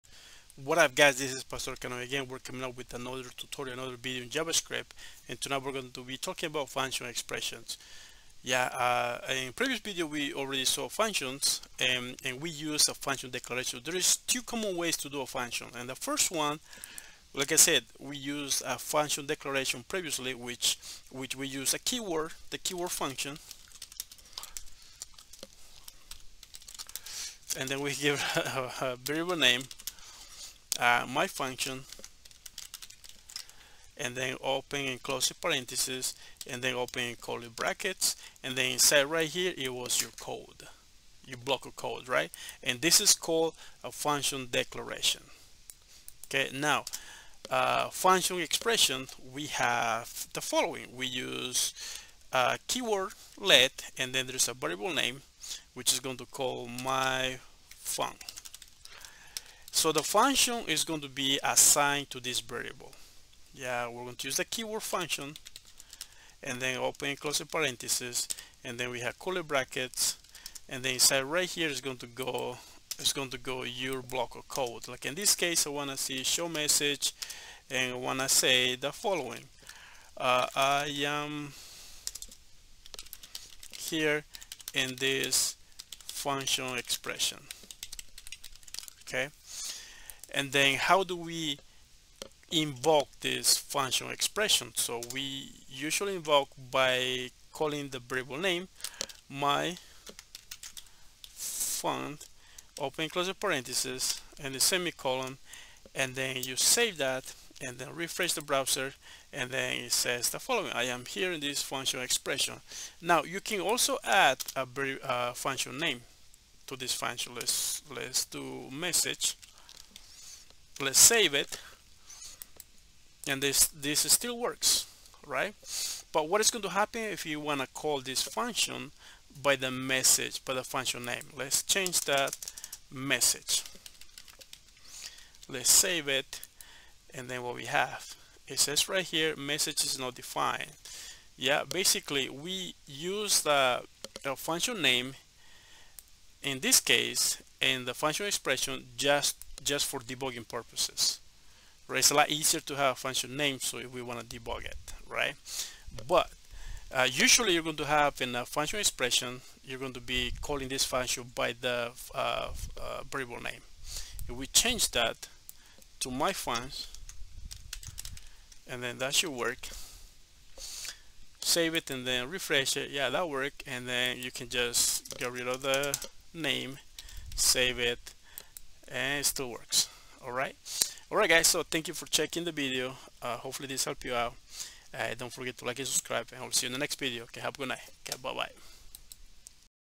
JAVASCRIPT TUTORIALS - FUNCTION EXPRESSION #22
This was a cool video to make about function expressions with JavaScript.  Hopefully this video helps you in your journey.
Thanks.